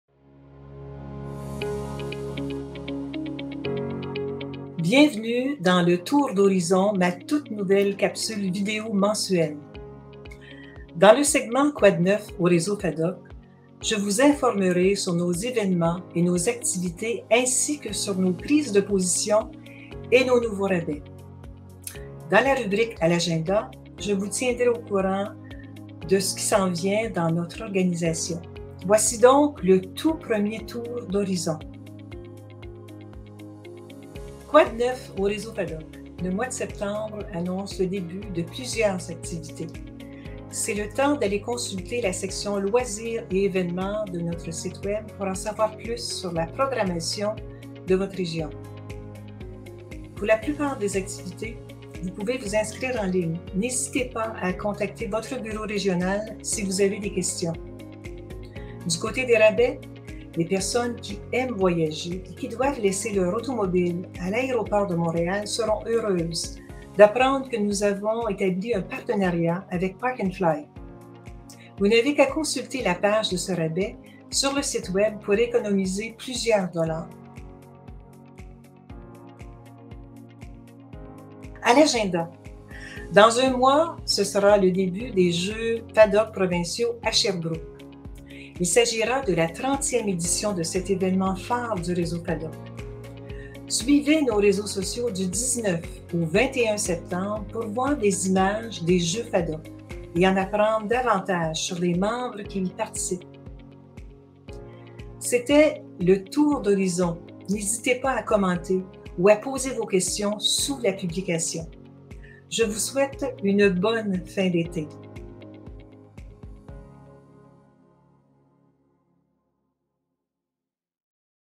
Le tour d'horizon de la présidente du Réseau FADOQ
Dans cette toute nouvelle capsule vidéo, Gisèle Tassé-Goodman vous parle du retour des activités, d'un nouveau partenariat avec Park'N Fly et des Jeux FADOQ provinciaux.